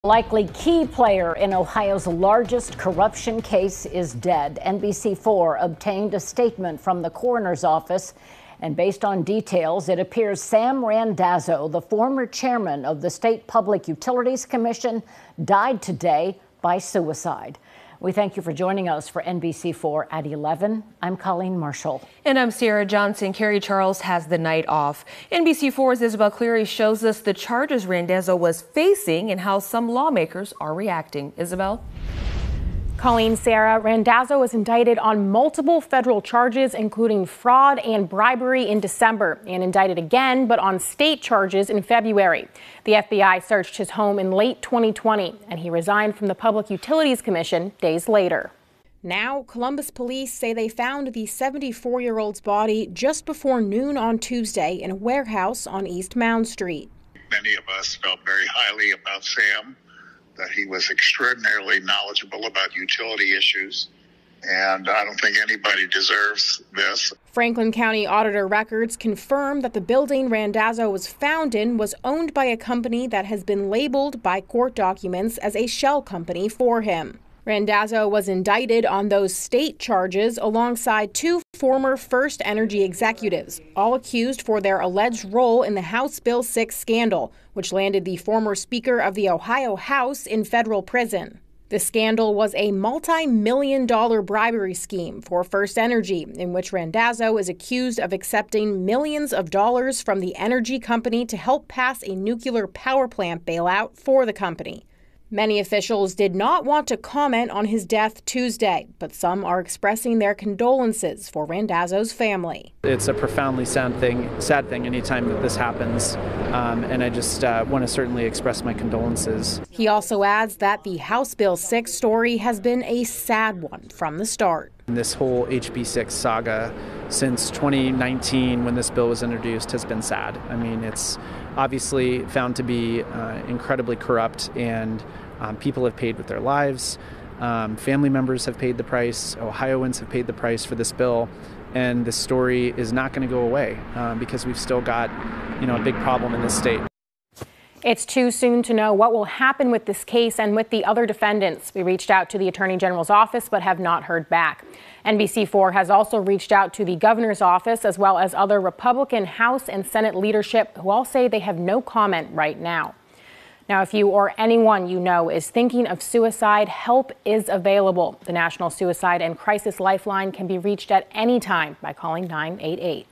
Sam Randazzo, recently charged in Ohio’s biggest corruption case, found dead
Ohio’s former top energy regulator, recently indicted as part of the state’s biggest corruption case in its history, was found dead Tuesday.